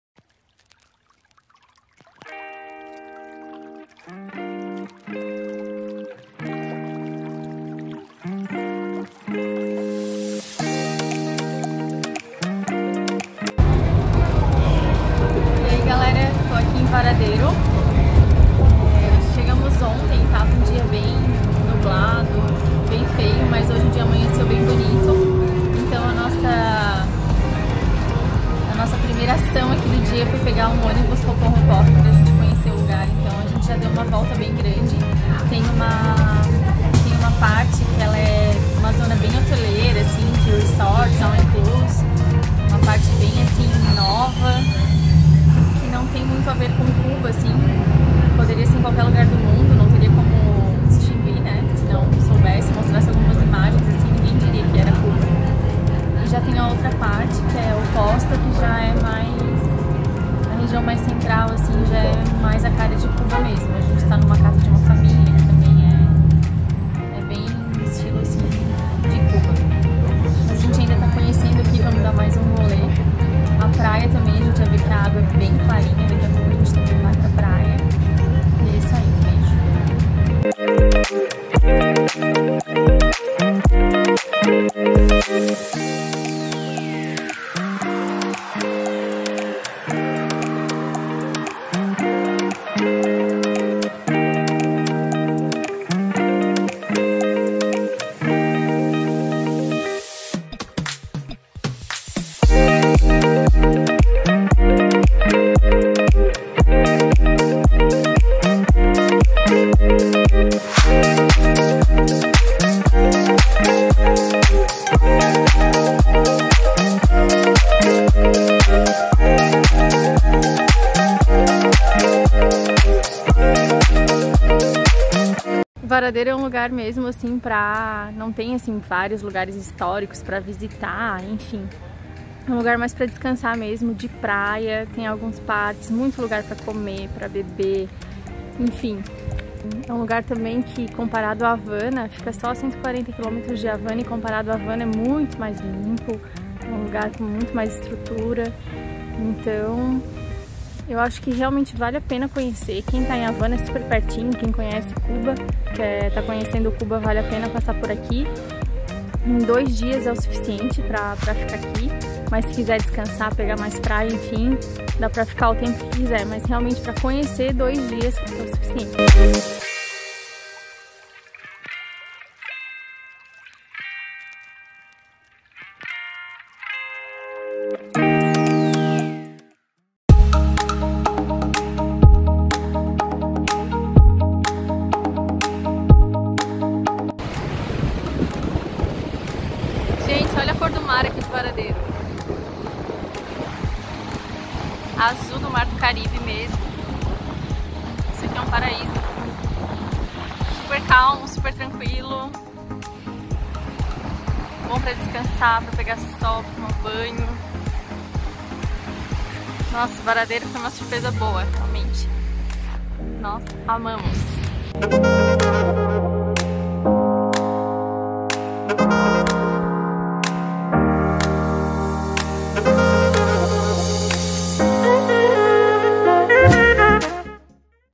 CUBA: VALE A PENA VARADERO? I CUBA 08
Vale a pena visitar Varadero?
Este é o oitavo vídeo da série Cuba do Viaje24h!